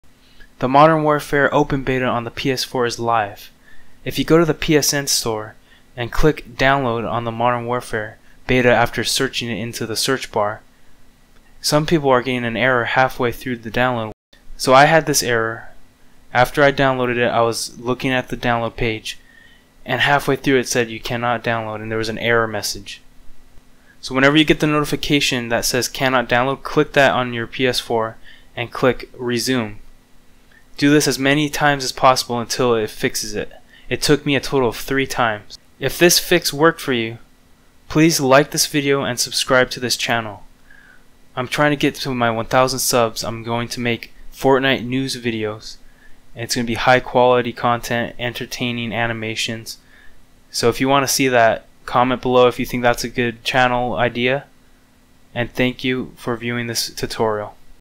Call of Duty Modern Warfare Beta Download Fix - PS4
A quick fix for the Modern Warfare Open Beta download error on a ps4.